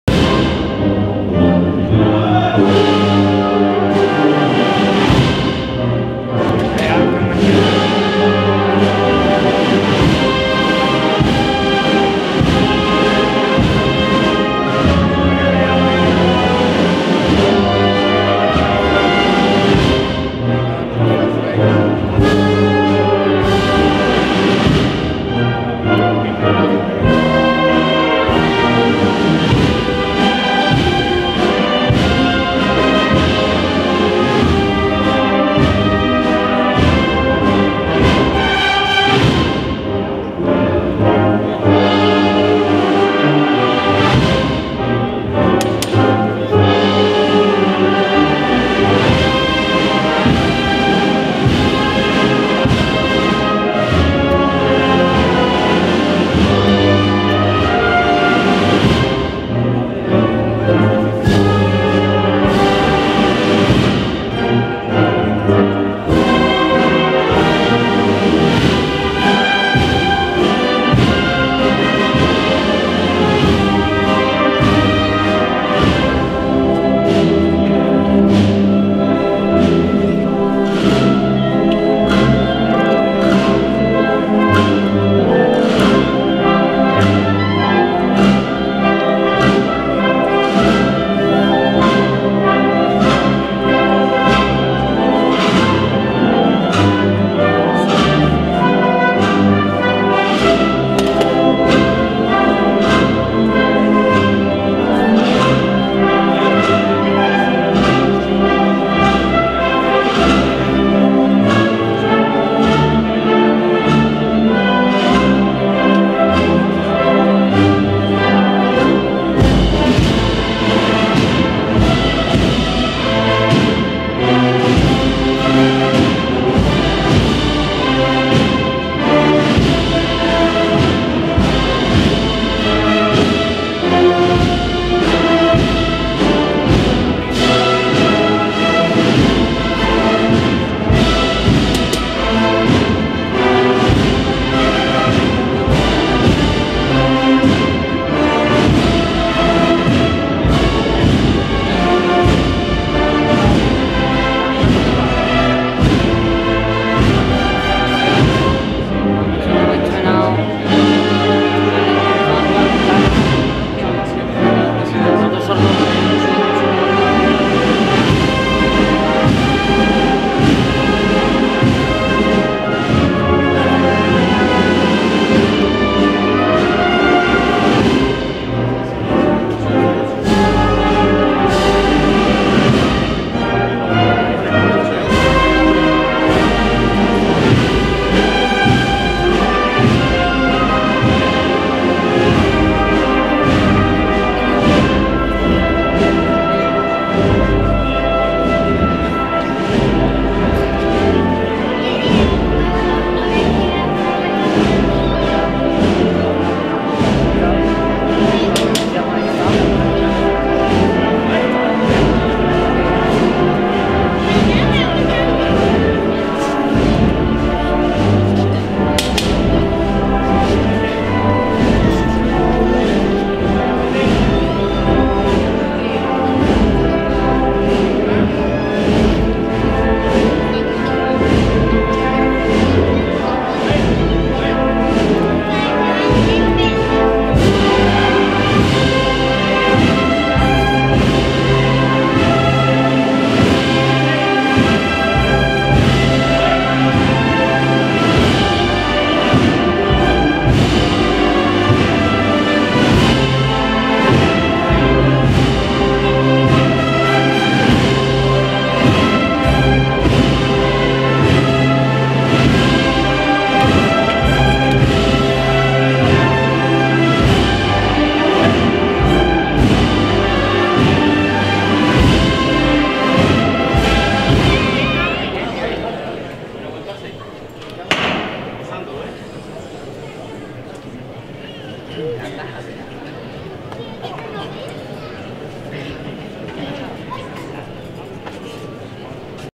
Procesión Nuestra Señora del Rosario de San Julian (Banda de Música Maestro Dueñas)
Sevilla 15/10/2017